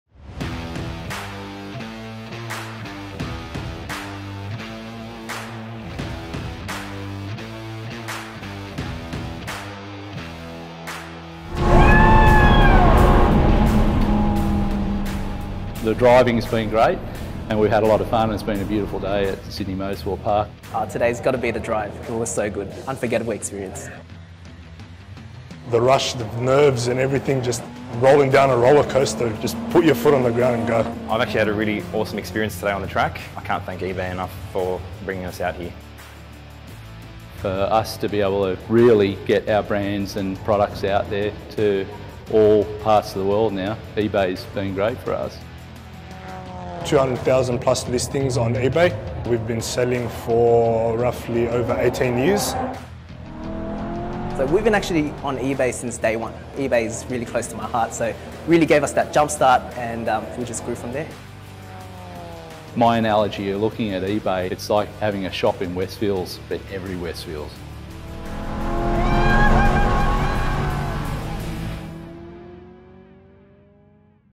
eBay Drive Day Wrap-up 2023 | eBay Selling
Last year, eBay took 30 of it's top Parts & Accessories sellers for a spin around the Eastern Creek race track in Sydney. We wanted to say thanks for all their hard work in 2023.

Watch our wrap-up video for a taste of what the day looked like!